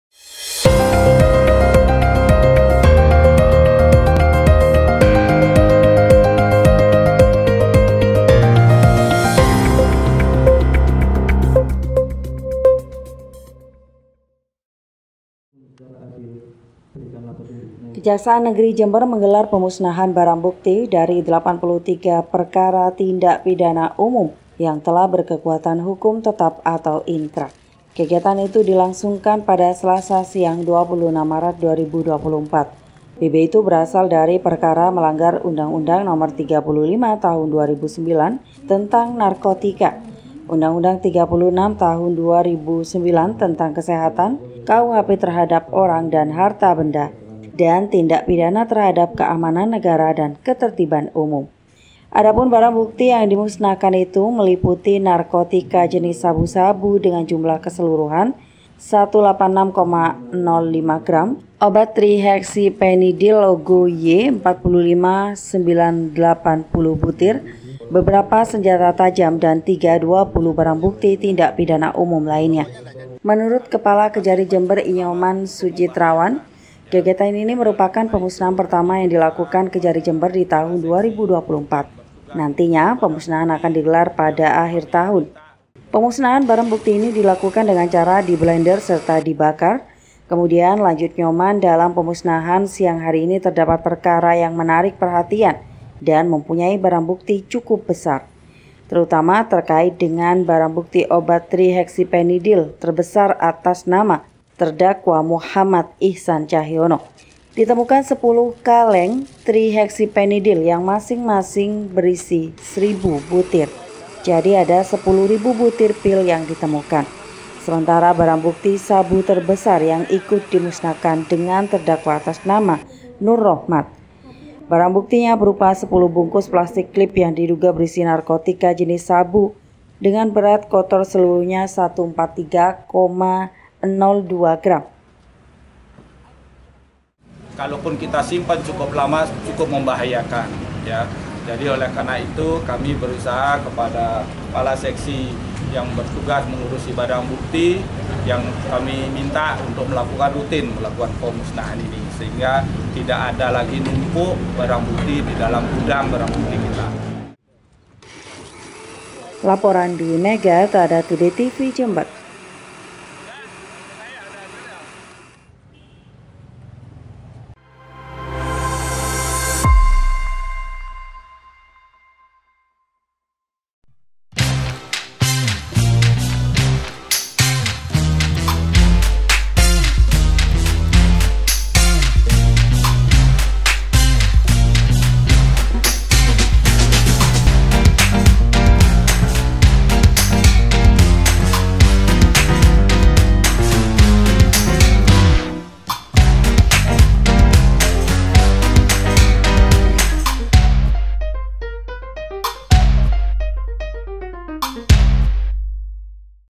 KEJARI JEMBER MUSNAHKAN RIBUAN BB DARI 83 KASUS
KEJAKSAAN NEGERI JEMBER MENGGELAR PEMUSNAHAN BARANG BUKTI/ DARI 83 PERKARA TINDAK PIDANA UMUM YANG TELAH BERKEKUATAN HUKUM TETAP ATAU INKRAH. KEGIATAN ITU DILANGSUNGKAN PADA SELASA SIANG 26 MARET 2024.